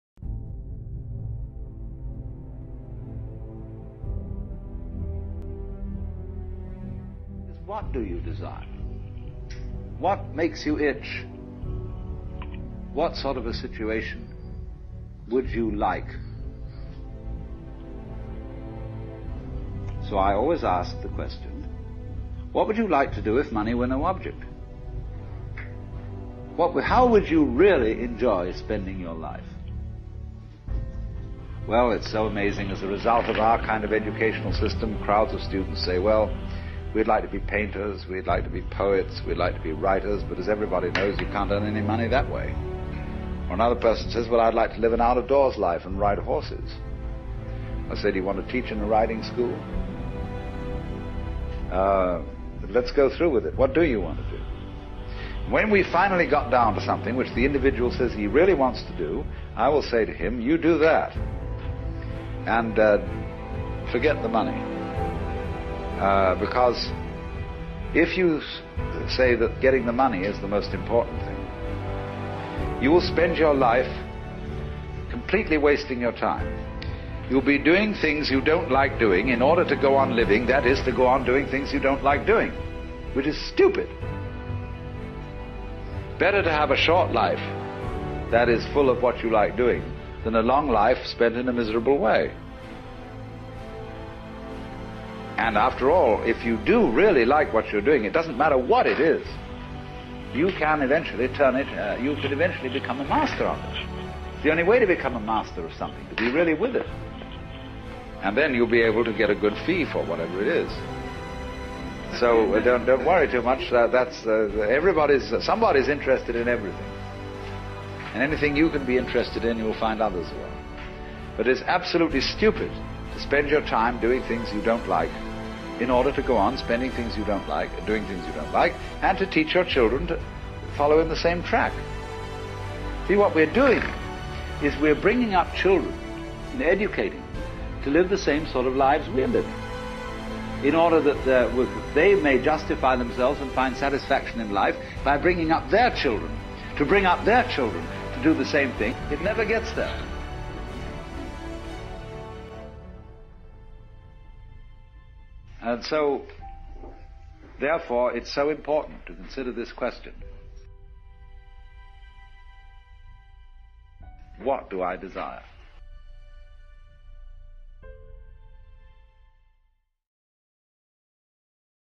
Alan watts- What do you desire? A short inspirational film
If you enjoyed this check out some of my newer films on my channel like,
Alan Watts - The Dream Of Life and More!! Cheers!
Speech by -Alan Watts
filmed on Dji Phantom 3, Gopro hero4+3
Filmed in Whistler blackcomb Canada, bansko Bulgaria, Cornwall UK
Uni Film